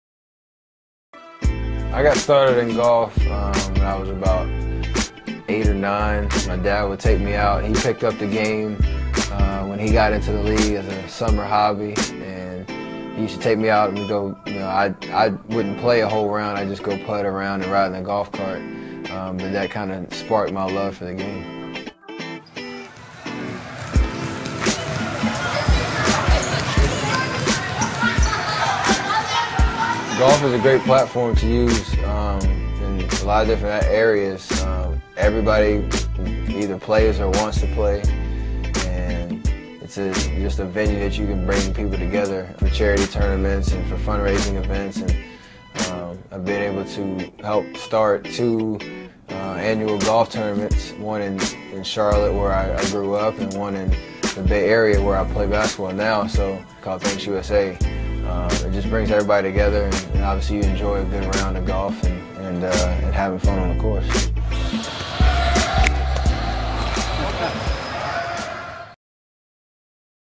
Steph Curry works on his golf game at Basketball City
Steph Curry working on his golf game at Basketball City